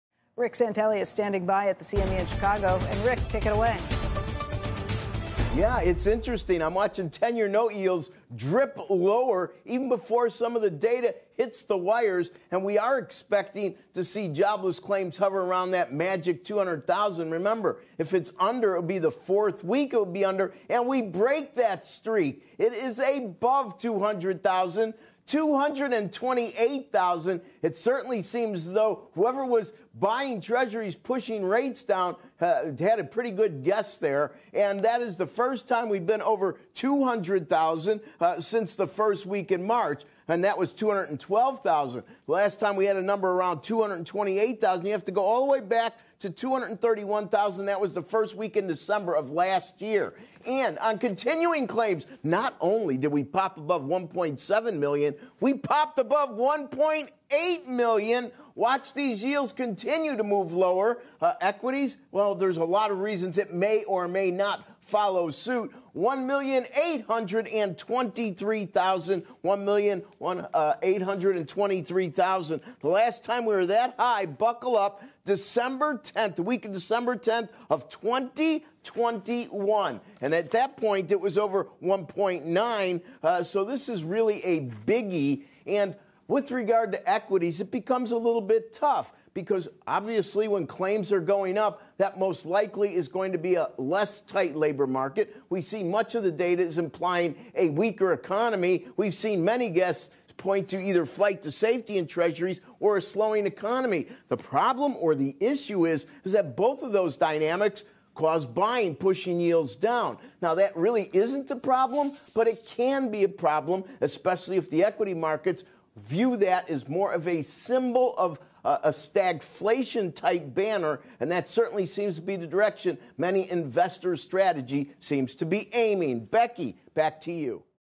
Initial jobless claims land at 228,000
CNBC's Rick Santelli reports on the latest economic data.